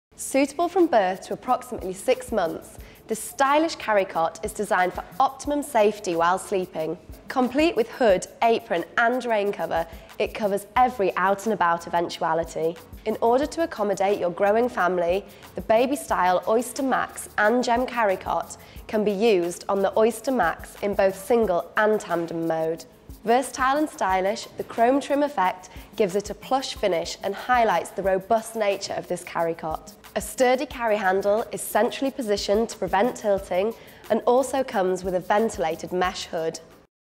Babystyle Oyster Gem Carrycot Kiddicare
View this video featuring the Babystyle Oyster Carrycot Colour Pack - Dolphin Grey product and shop other similar products on Kiddicare.com. - Products featured in the video: 1 Babystyle Oyster Carrycot Colour Pack - Dolphin Grey 2 Babystyle Oyster Carrycot Colour Pack - Lime 3 Babystyle Oyster Carrycot Colour Pack - Tomato 4 Babystyle Oyster Carrycot Colour Pack - Electric Blue
Change the look of your pram to suit your mood!

The BabyStyle Oyster Carrycot Colour Pack enables you to change the appearance of your Oyster carrycot to suit you The Colour Pack includes an apron colour co-ordinating hood provided in a matching re-usable carry-bag.

FEATURES
Change the look of your oyster carrycot
Pack includes Apron and hood